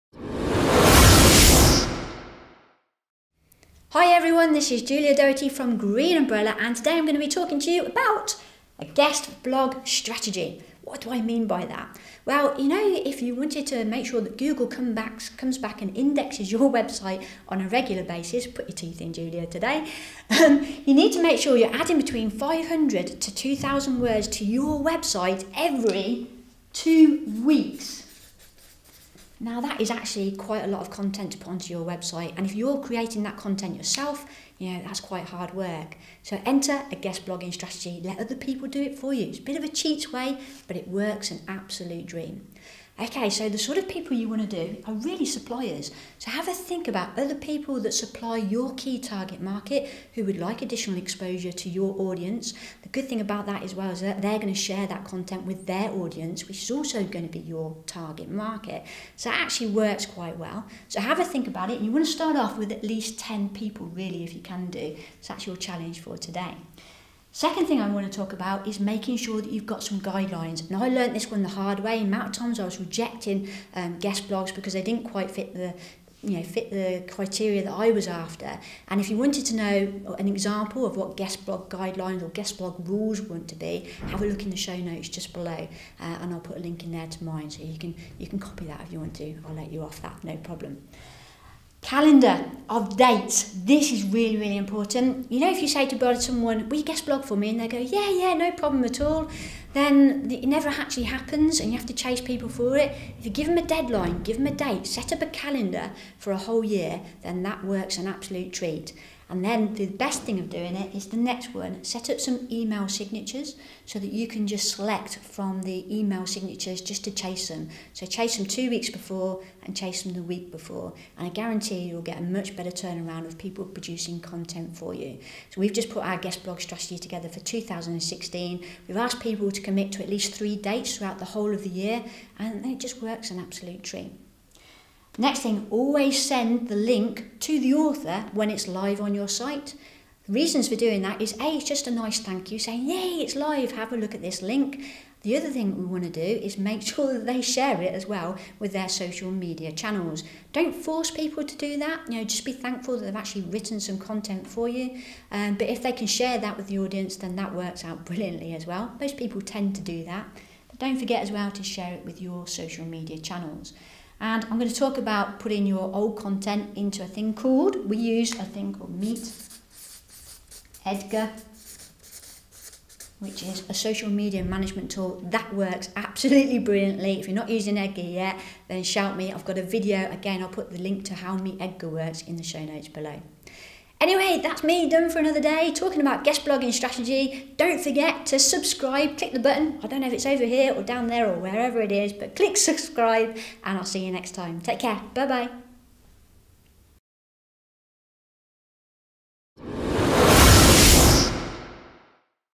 Why a Guest Blogging Strategy is The New Black
We all know that adding new, unique content to your website is paramount nowadays if you want your website to rank well in Google. Writing content  multiple times a week on your website is hard work.  It is extremely difficult to produce good quality content week after week, so consider using a guest blog strategy for your site.

What is a guest blog strategy?
Think of your referral network.  Who do you know that is either a good referral partner for your business or other suppliers to the same industry?  Make a list of potential guest bloggers who you think would like exposure to your target market in exchange for content for your website?

The key to success for a guest blogging strategy is:-

1. Set out the rules (here is our guest blog guidelines for the Green Umbrella

2. Allocate a date for your bloggers. If you do not give them a deadline then it will rarely happen.

3. Chase them with standard emails (we have signatures set up).

4. Always send them the link to the article for their approval before you add to your social media channels. This nudges them to also share the article with their own network.

5. Always tag them in a Tweet, Facebook or LinkedIn profile, as I guarantee that they will RT or comment on the post, which adds additional exposure.

If you want to find out more about blogging strategies, then download a free Blogging Checklist